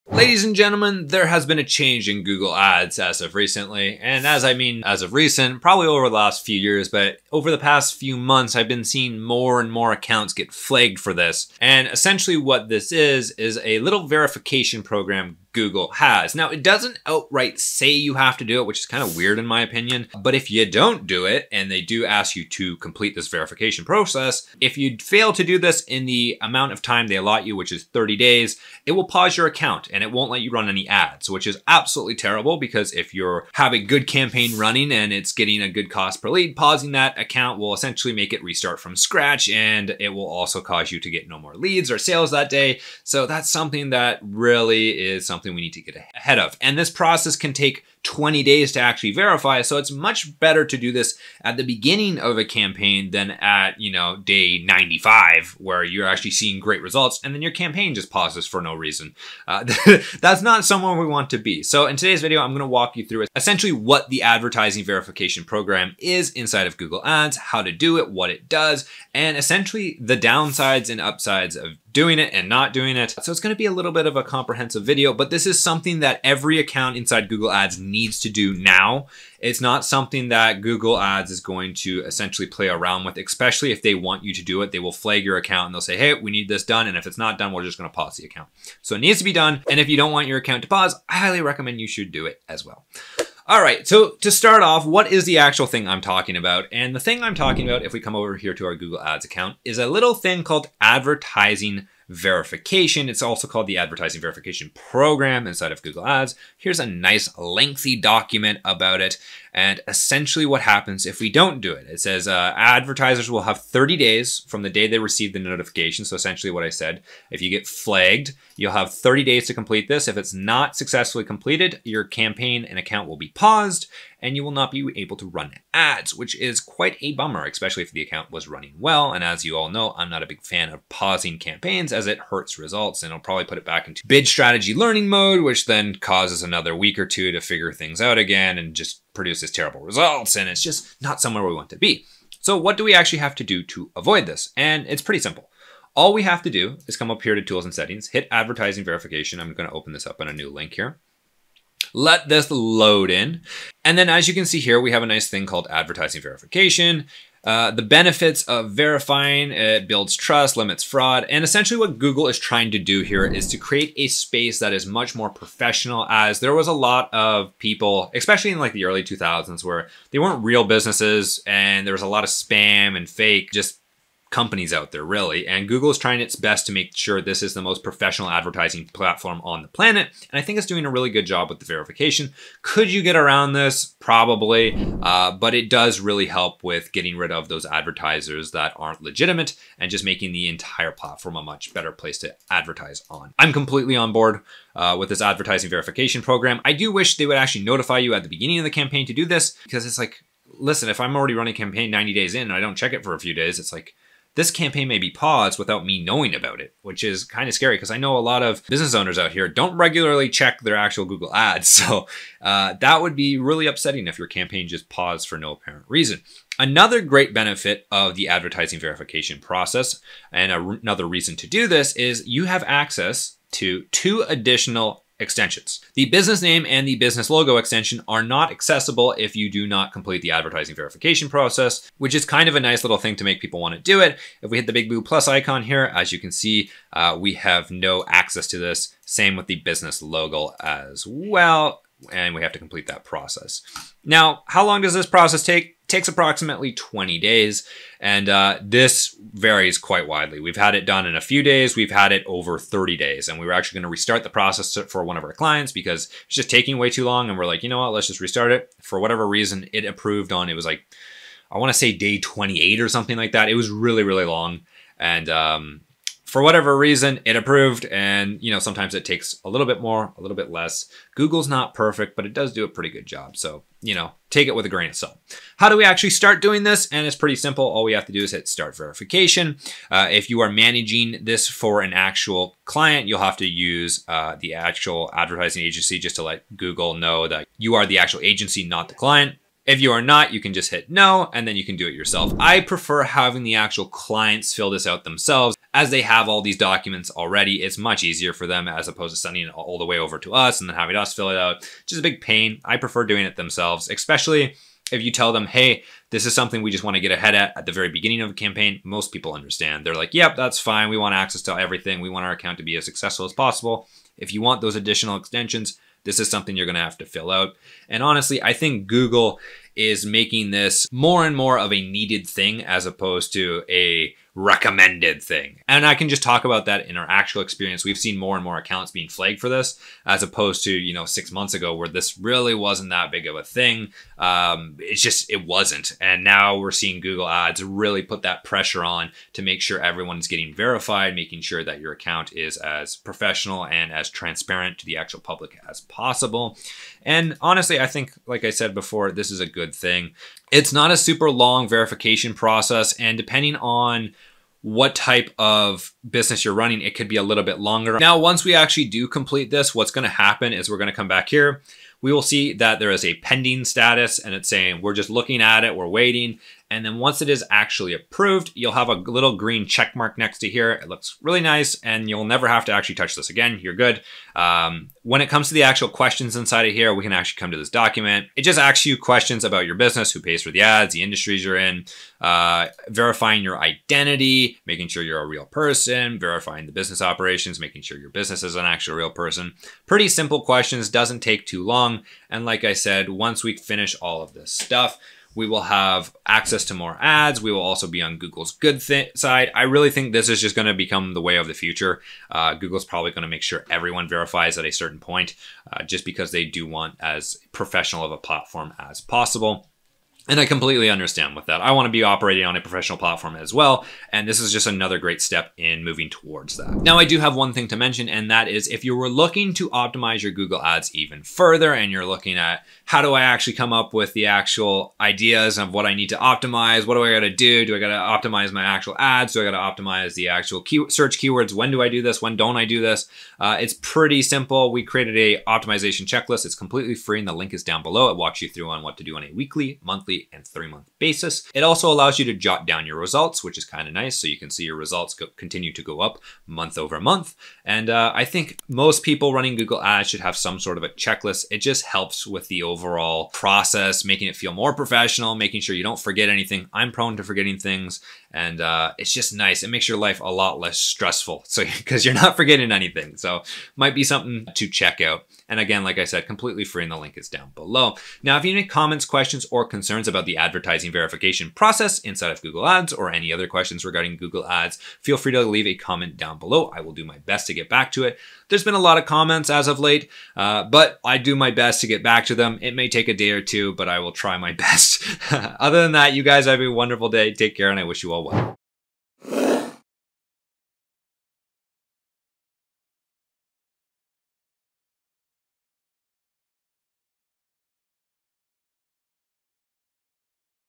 Google Ads Advertiser Verification Explained (And Why You NEED To Do It Now)
Are you ready to take your Google Ads campaigns to the next level? In this comprehensive guide to Google Ads Advertiser Verification, we delve into the essential steps and details you need to know to get verified and optimize your advertising strategy!

🌟 What is Advertiser Verification? 🌟

Advertiser Verification is a process implemented by Google to ensure transparency and trust between advertisers and users. It helps establish legitimacy and credibility for businesses running ads on Google's platform.

🔑 Key Benefits of Advertiser Verification: 🔑

Improved credibility and trust with your audience.
Enhanced ad performance and visibility.
Strengthened brand reputation and authenticity.

Step-by-step guide to the Advertiser Verification process.
Tips and best practices to streamline the verification procedure.
Insights into why verification matters for your advertising success.
Real-world examples of successful campaigns post-verification.

🎓 Who Can Benefit from This Video? 🎓

Business owners and marketers leveraging Google Ads.
Agencies managing advertising for clients.
Anyone seeking to maximize their advertising impact on Google's platform.

🚀 Join us as we navigate the ins and outs of Google Ads Advertiser Verification, providing you with the tools and knowledge to elevate your advertising game! Don't miss out on this opportunity to stand out and thrive in the digital advertising landscape.